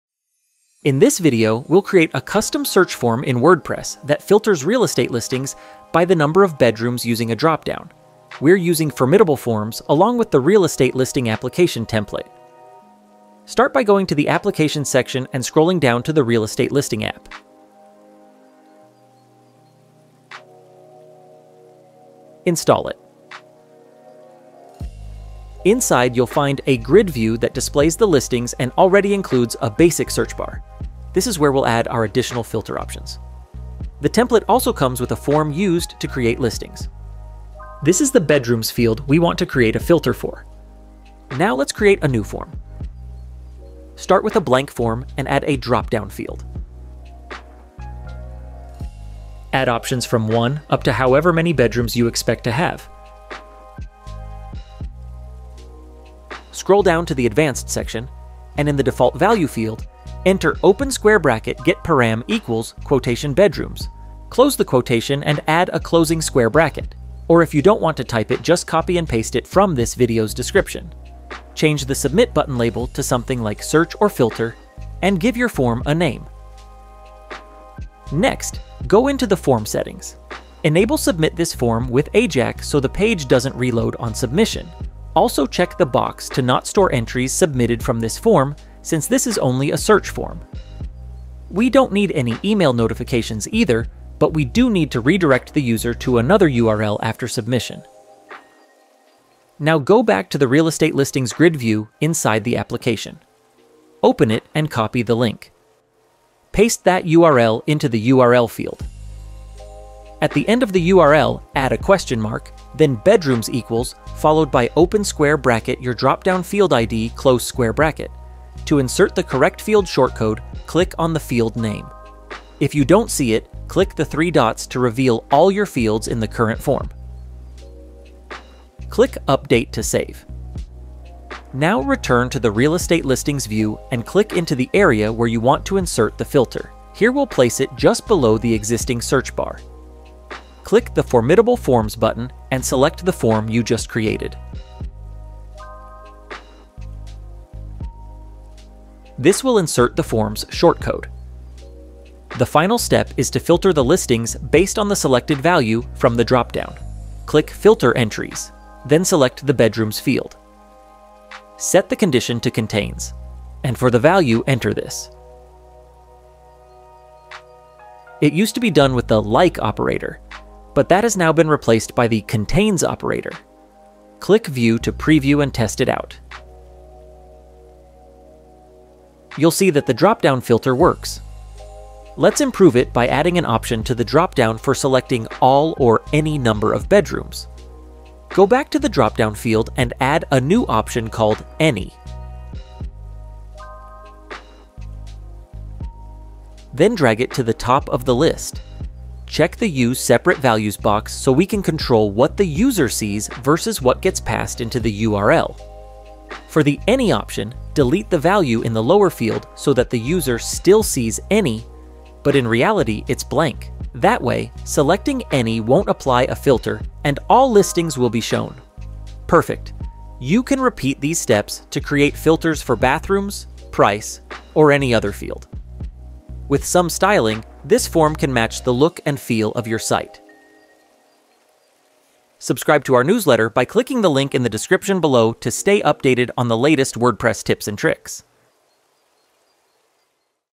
How to Create a Searchable Database in WordPress with Dropdown Filters
Learn how to create a custom search form in WordPress with dropdown filters using Formidable Forms. Perfect for real estate listings, directories, or any site with searchable content. No coding needed!

Here is the parameter used in the video (you don't need to call it bedrooms):
[get param="bedrooms"]

00:00 Intro
00:14 Install the Application Template
00:45 Building the Search Form
02:12 Adding the Search Form to the view
02:58 Adding an "Any" option